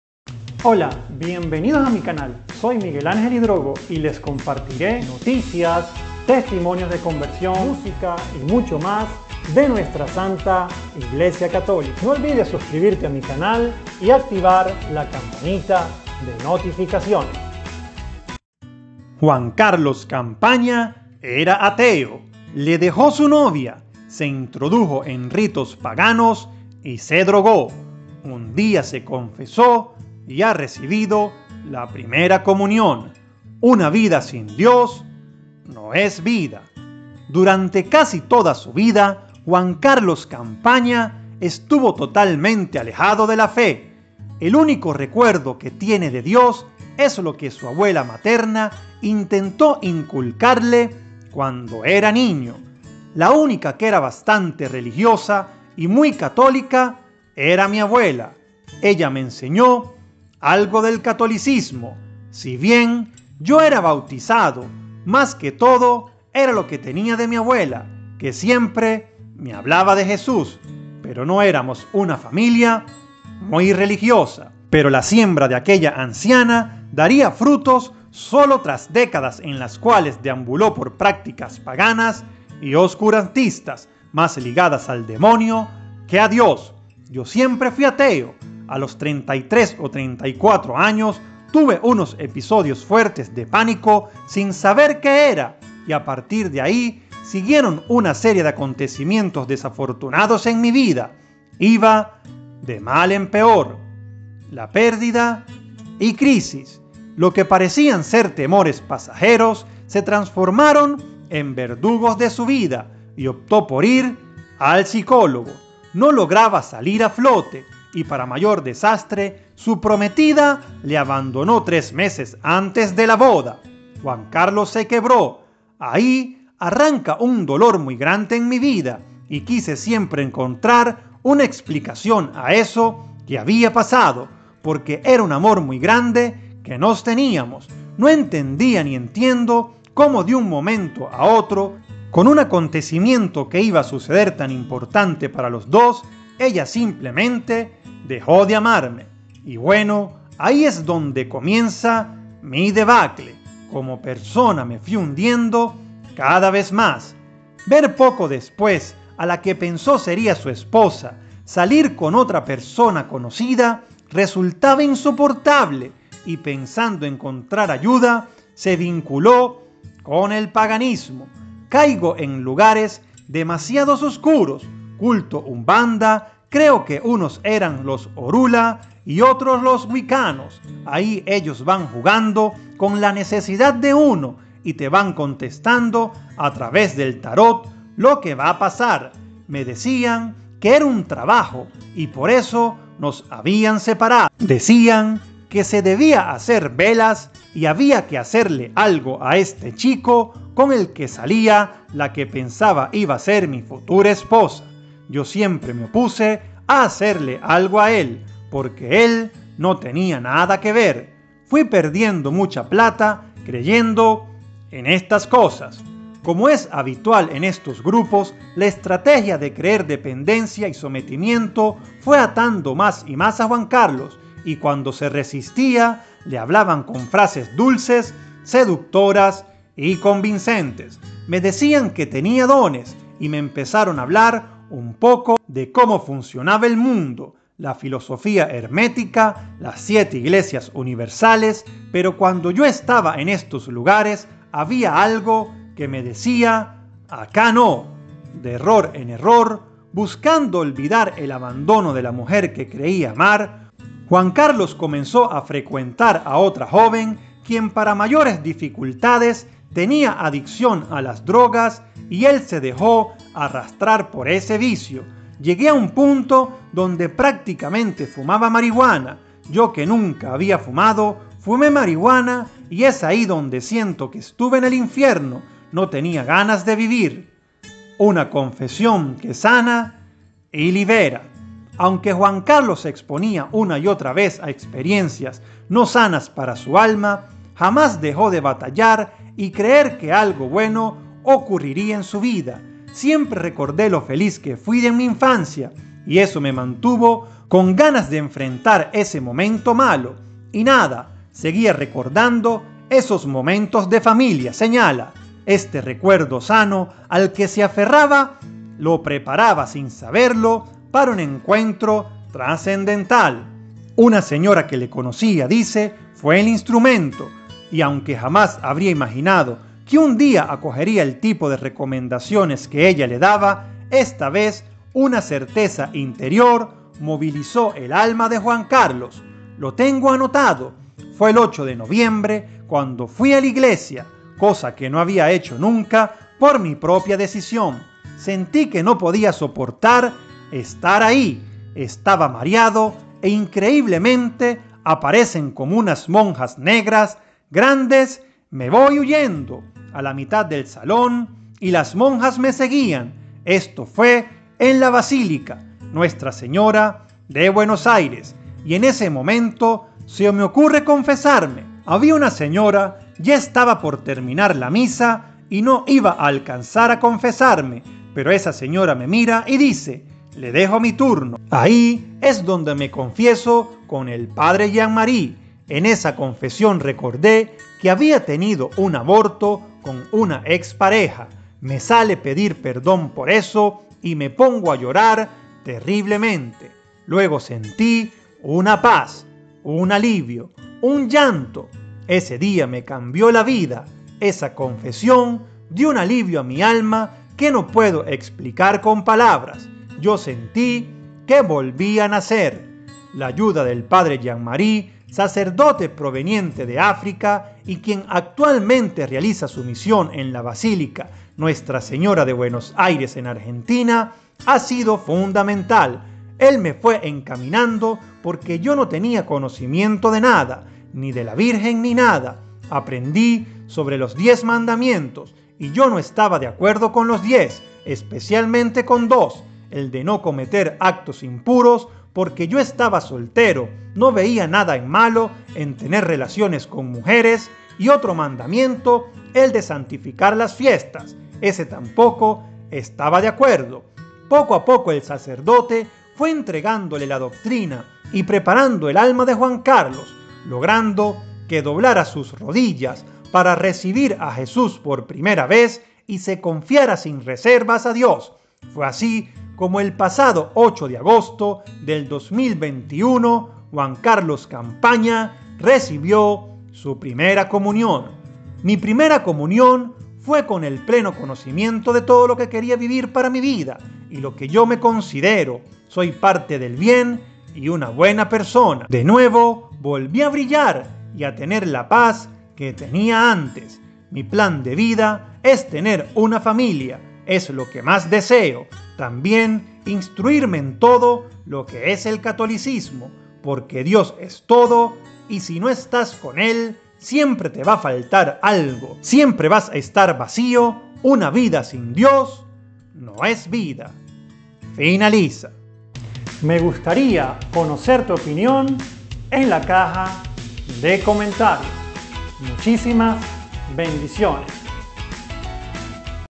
ERA ATEO Y OSCURANTISTA se confesó con un SACERDOTE y CAMBIÓ SU VIDA
Juan Carlos Campaña era ateo, le dejó su novia, se introdujo en ritos paganos y se dr0gó, un día se confesó y ha recibido la Primera Comunión: «Una vida sin Dios no es vida»

fuente: Caminocatolico.com